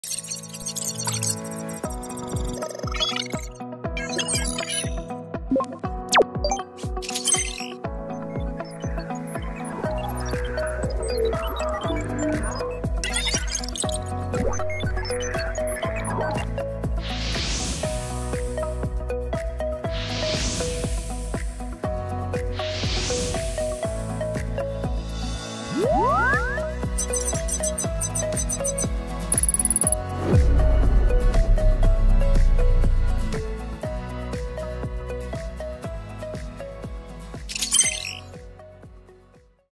Orbs Teaser
The Future of WE, AGENT is Here.

With innovation and collaboration at the core, we’re thrilled to share a teaser of our ready-to-release new AI agent, the Orbs, that gives you a sneak peek of what's coming next. 🚀